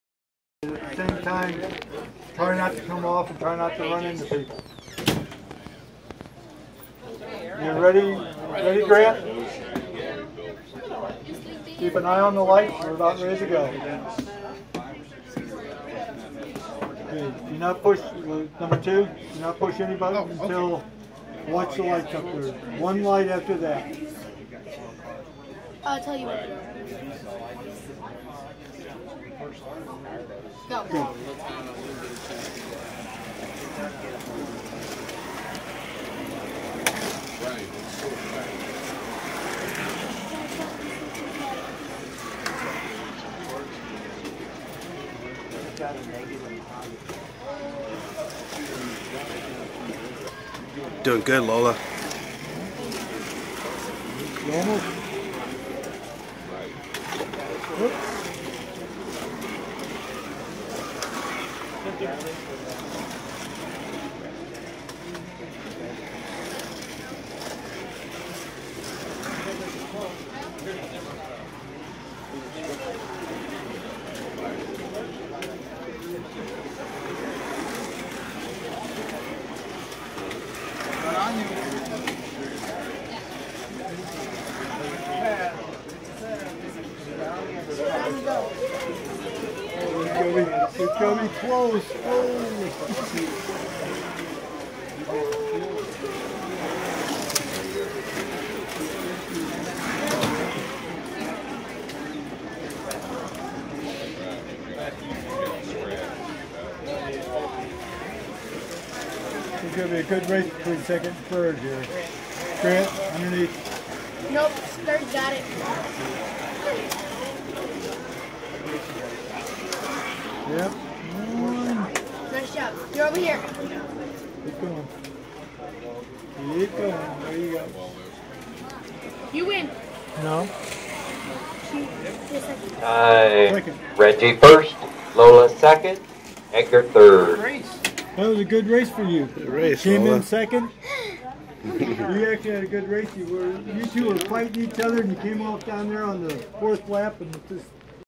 Slot car racing at hobby store. Lola gets second!
Our local hobby shop has slot car races every Friday night. Lola wanted to give it a try and she got second place in her first race! It was such a great time...